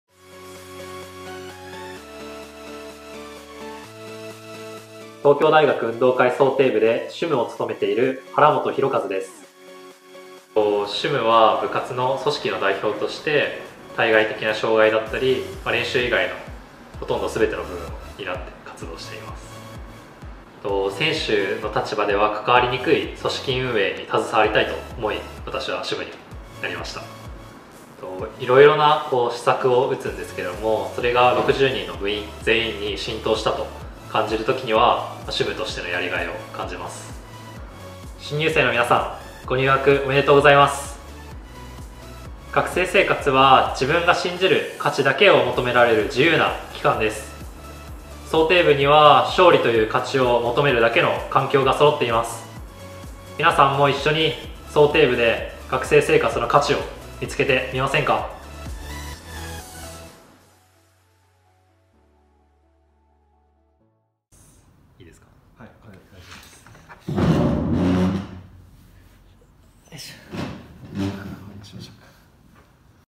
東京大学運動会 漕艇部 主務インタビュー 2021
東京大学運動会漕艇部の2021年度主務、原本さんのインタビュー映像です。

運営の要として部に携わっている原本さんの部に対する思いや新入生に対する言葉、グッとくるものがあります✨